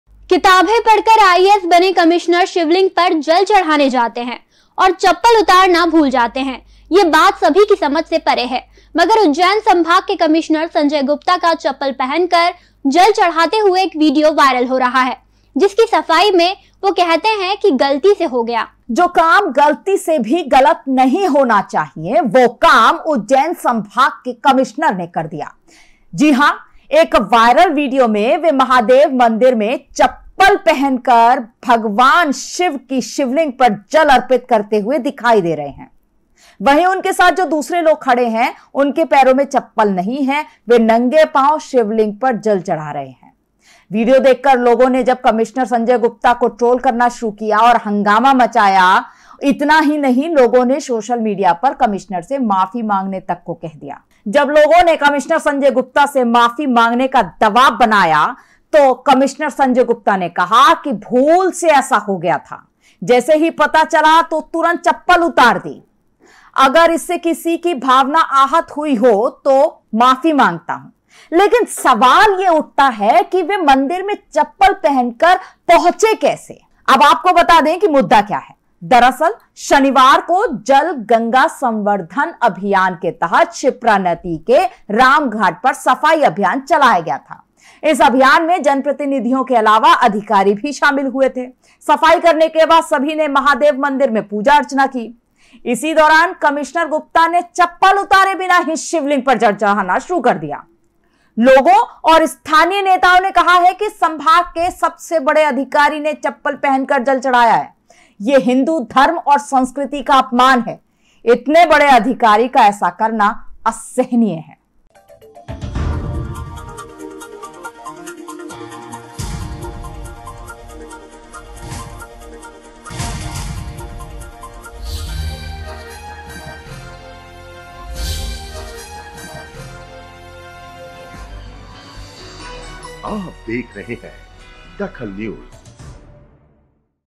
DAKHAL NEWS कमिश्नर ने चप्पल पहनकर शिवलिंग पर चढ़ाया जल
कमिश्नर ने चप्पल पहनकर शिवलिंग पर चढ़ाया जल
वीडियो हुआ वायरल मच गया हंगामा मांगी माफ़ी